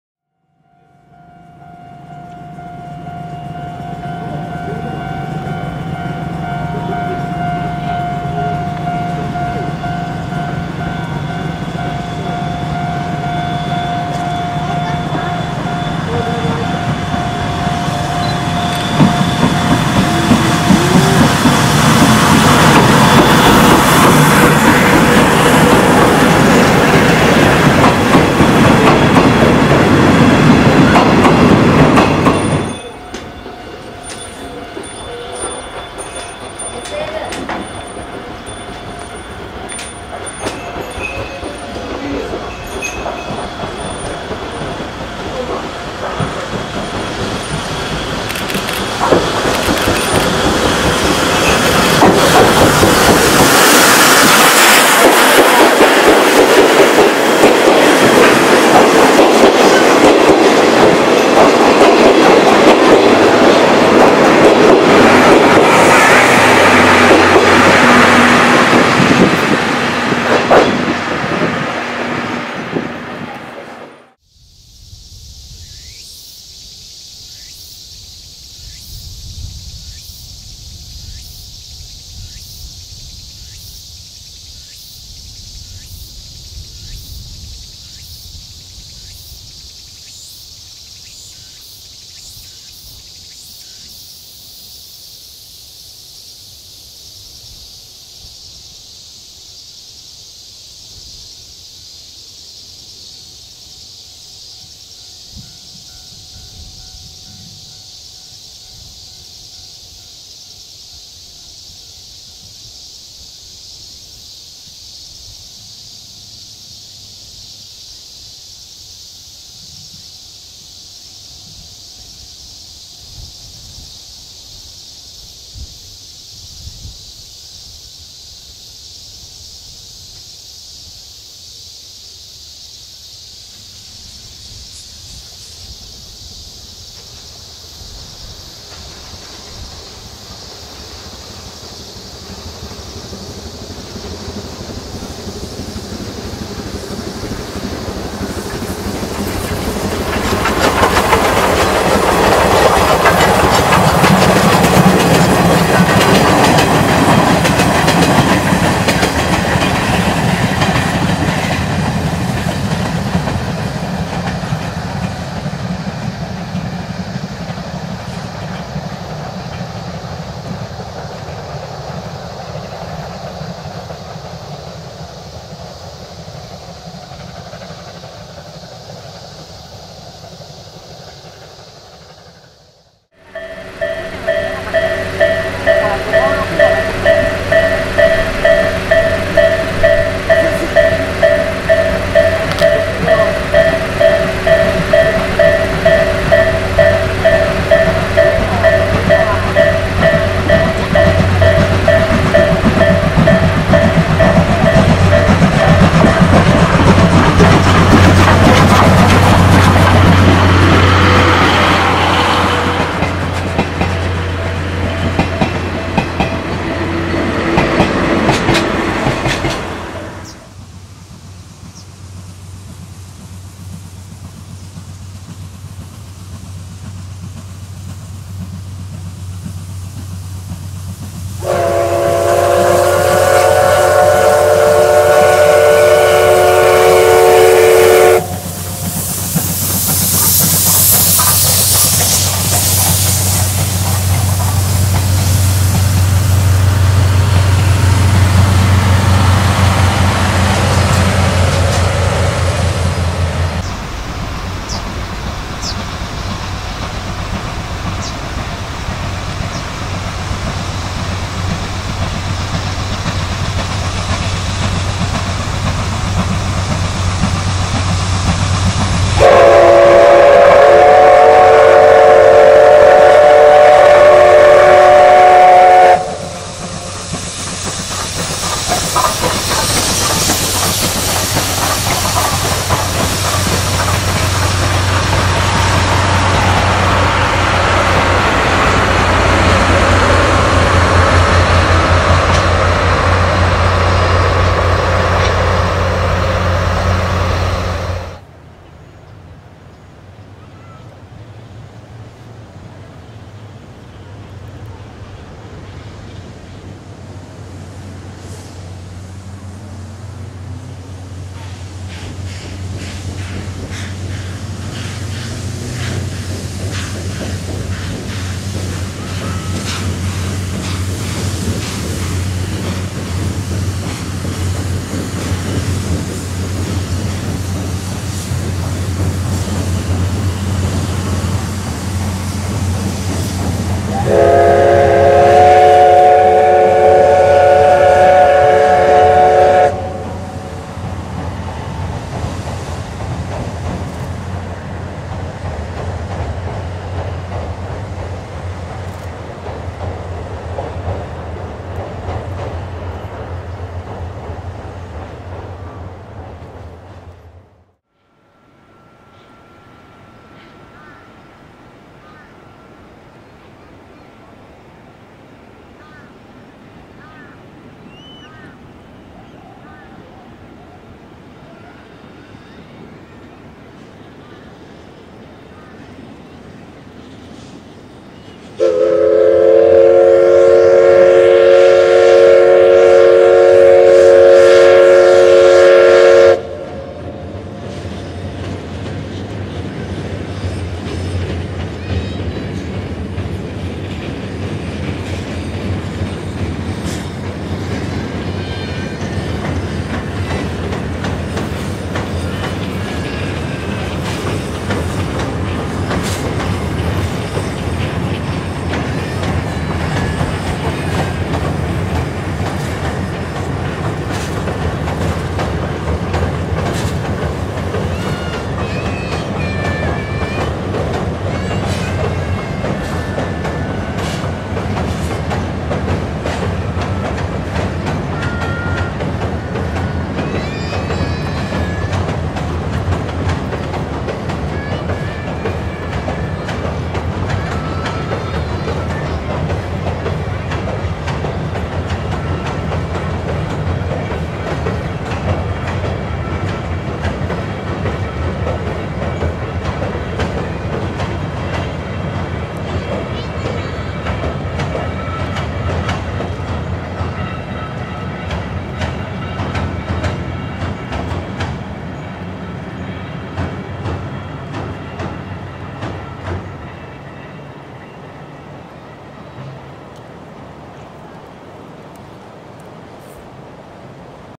全国私鉄SL列車 秩父鉄道・真岡鉄道・東武鉄道・大井川鉄道
①秩父鉄道　SLパレオエクスプレス　2016年4月撮影
②真岡鉄道　もおか号　2017年8月撮影
③東武鉄道　SL「大樹」 2017年8月撮影
④大井川鉄道　トーマス号 2014年9月撮影

撮影機材
・SONY FDR-AX55
・SONY HXR-NX3

SNS